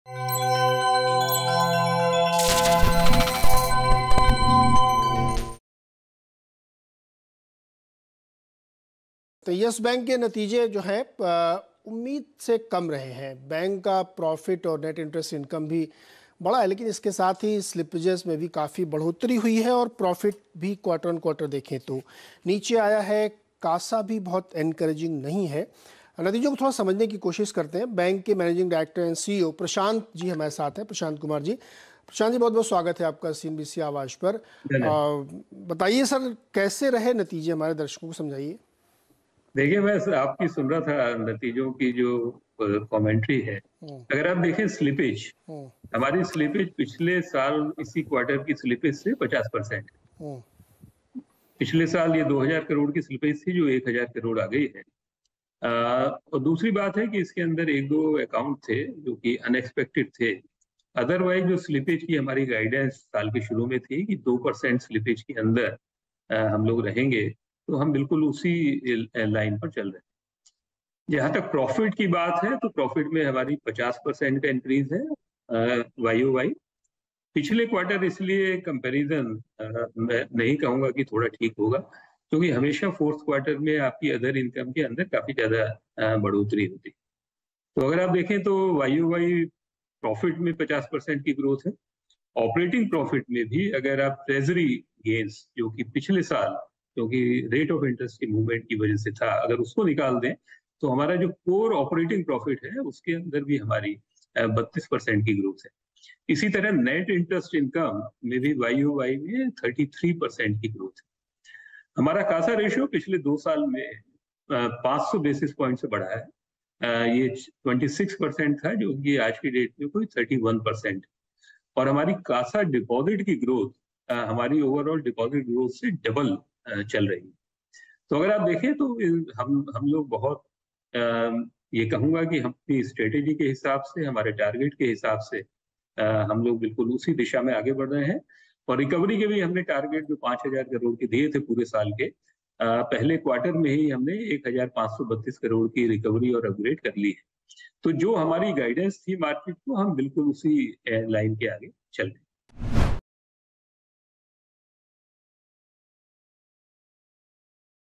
Yes Bank के शेयरों में क्यों मचा कोहराम, मैनेजमेंट ने बताया सच
Yes Bank के Q1 के नतीजे सामने आ गए हैं. Slippage पर क्या है Management का कहना? कैसी है Company की Business Growth, Revenue, इन सभी पर Management संग चर्चा